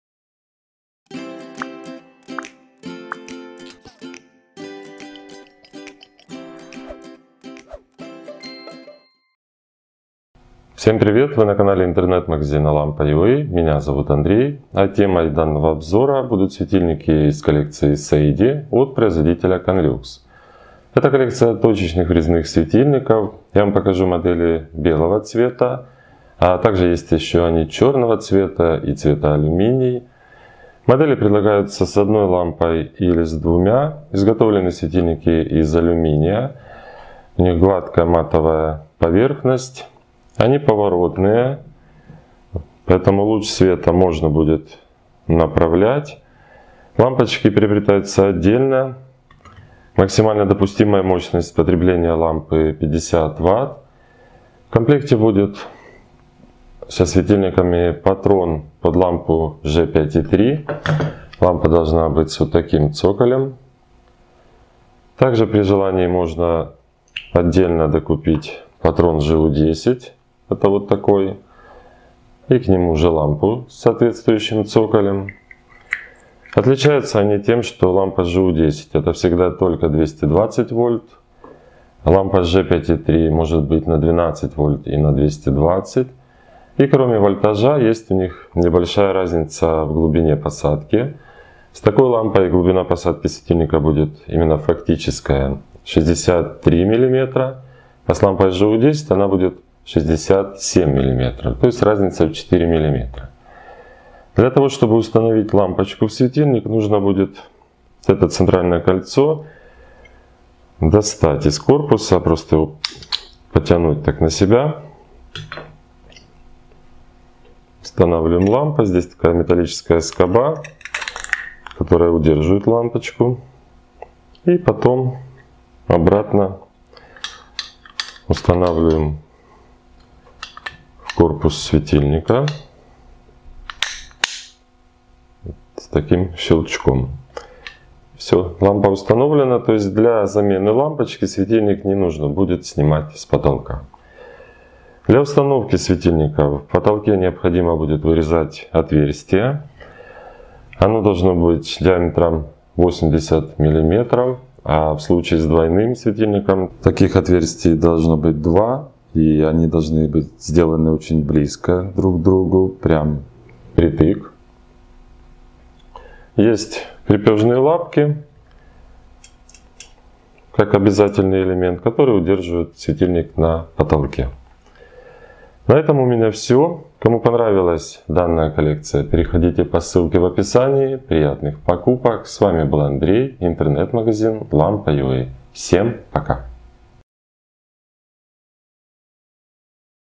Точечный светильник KANLUX 95251, 95250 (KANLUX 19455, 19454 Seidy) обзор
Купить точечный врезной LED светильник на две лампы KANLUX 95251, 95250 (KANLUX 19455, 19454 Seidy)

Обзор светодиодного светильника квадратной формы в стиле хай-тек из коллекции Seidy от бренда KANLUX (Польша)

Из ролика вы узнаете про:
- другие светильники из коллекции;
- материалы светильника;
- технические характеристики;
- максимальную мощность потребления светильника;
- рассеивание освещения;
- диаметр крепления;